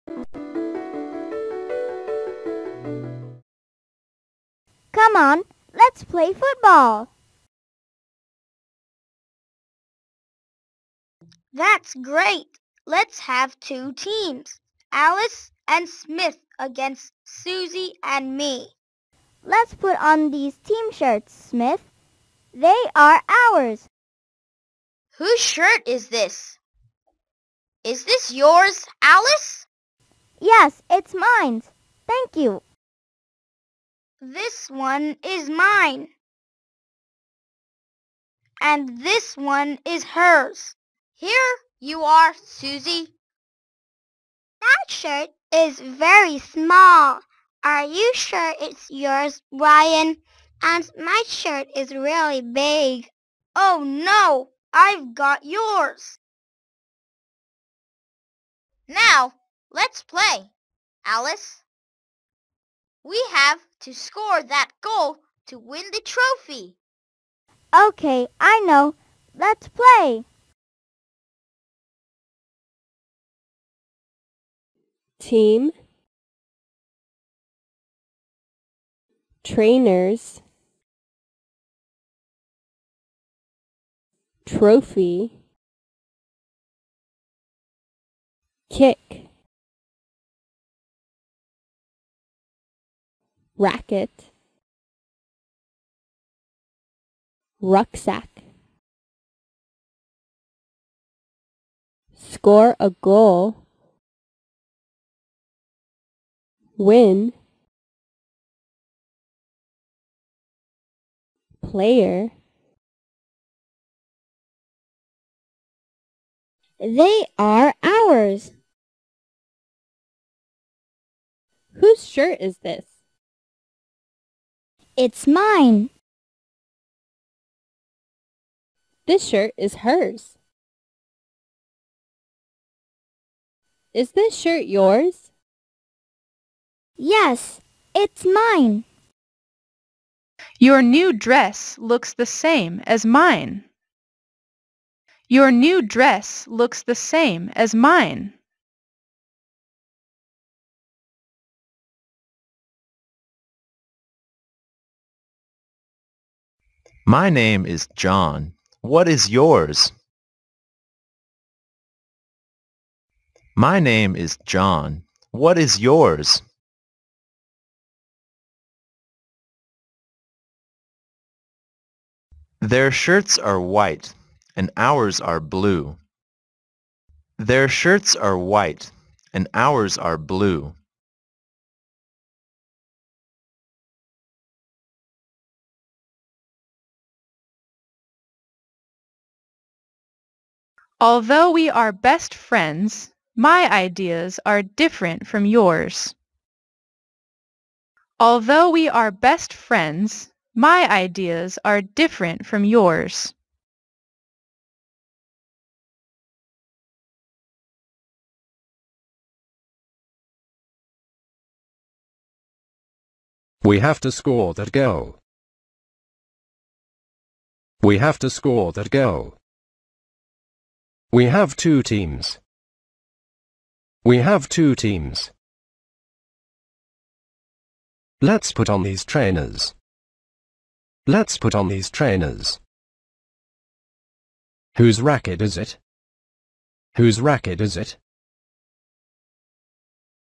Hello Felix | CVL Unit 14 Let's play football - Learning english for kids
Hello Felix | Conversation & Vocabulary & Listening
 (CVL) Unit 14 Let's play football - Learning english for kids

The purpose of unit 14 is to help 10 years old kids to know fundamental rules of king sport.

You are watching video of CVL Unit 14 Let's play football on Hello Felix  - online English teaching channel for children on Youtube !!!